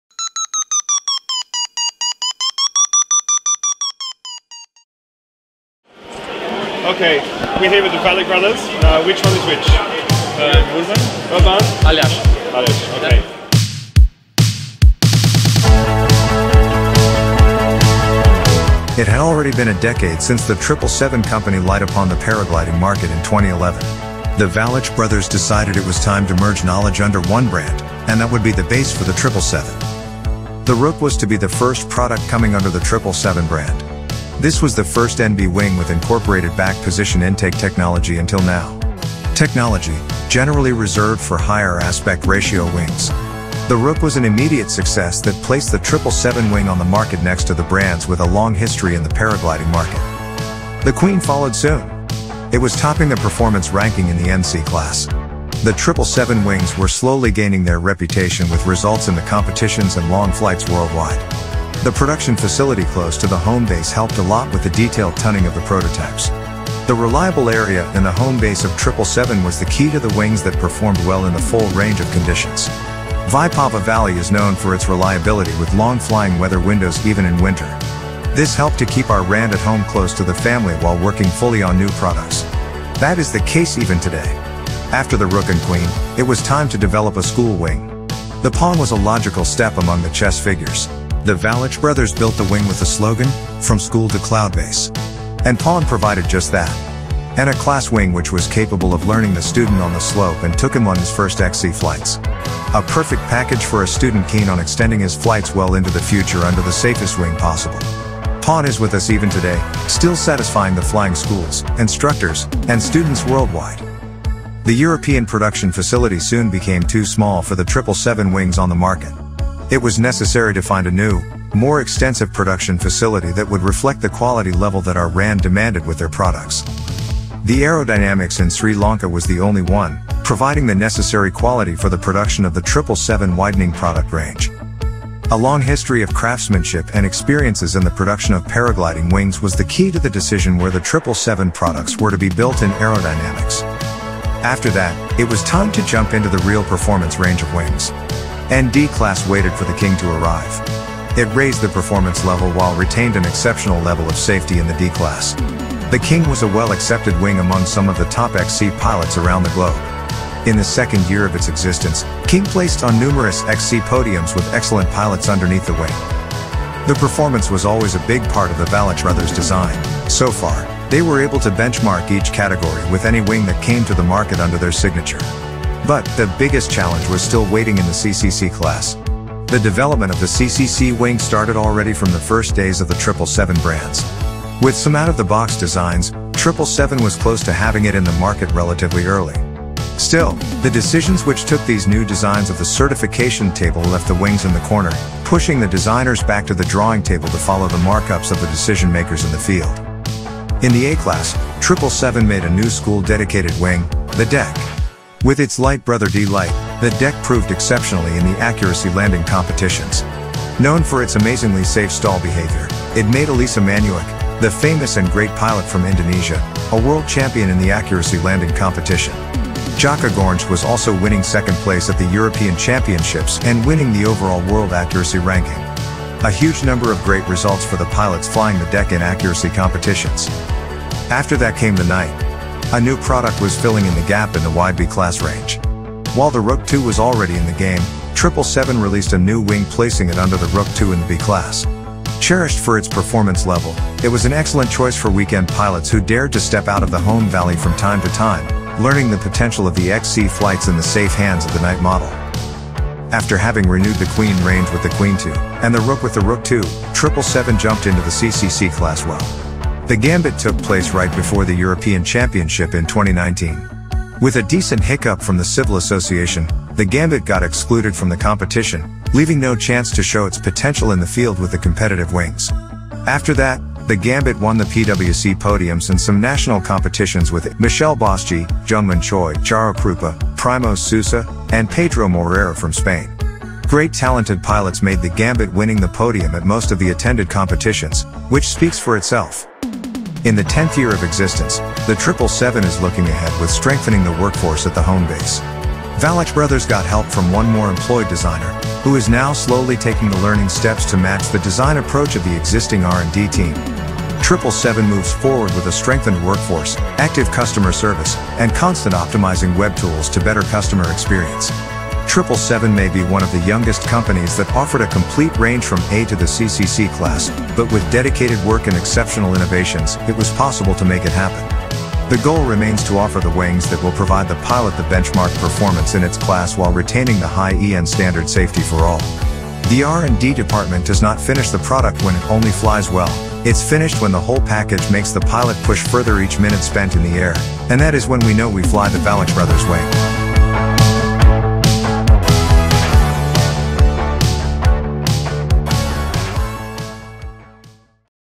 Triple Seven - Celebrating 10 years
Jump on a historic ride with us! Triple Seven celebrates 10 years.